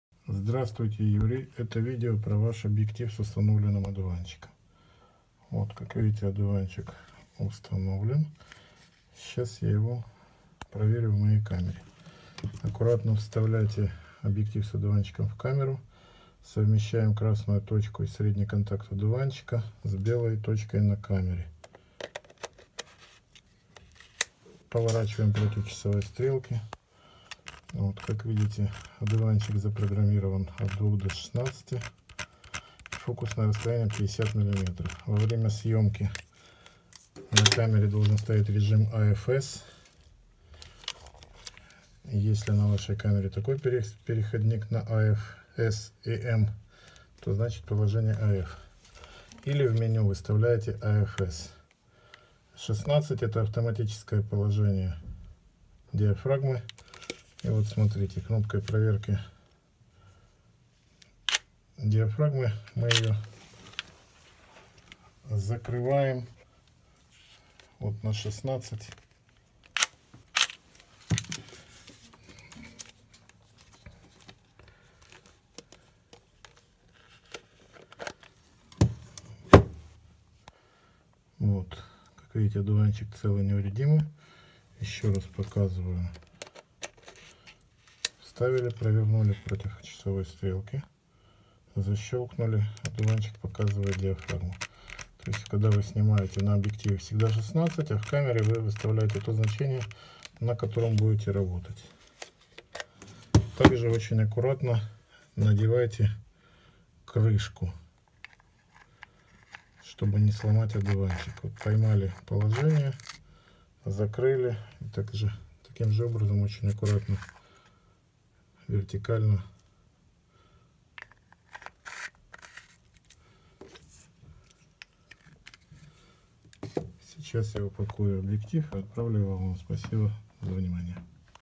Для Юрия П.
МС Гелиос-81-Н с установленным Одуванчиком Лушникова для Nikon.